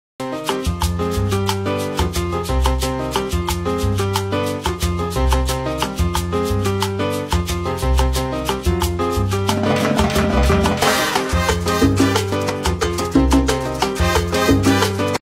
How to save money when using your oven & hob
Follow these tips to bring down your bills ✅